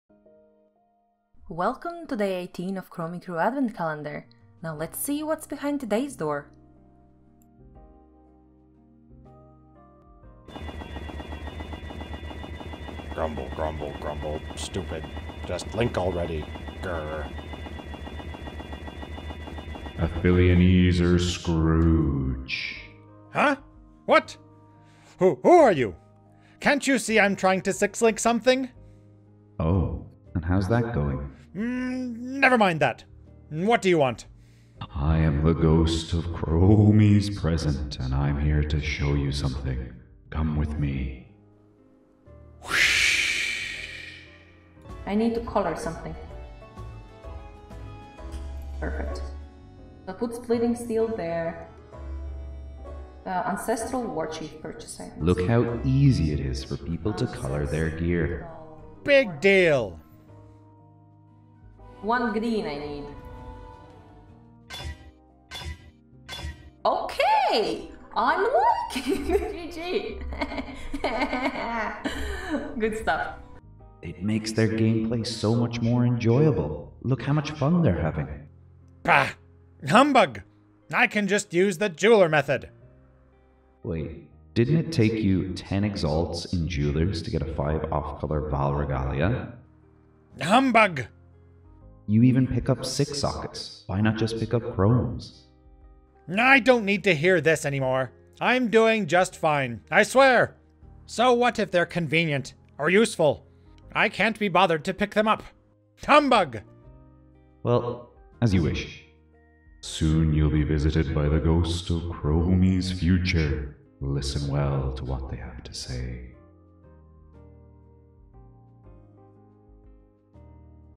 Chromie Crew Advent Calendar Day 18 - Audiobook: A Chromie Christmas Tale Part 2
Hello, hello, hello! Welcome back to part 2 of Chromie Christmas Tale - Aphelioneezer Scrooge meets the Ghost of Chromies Present!

Please go tell them what a wonderful job they did!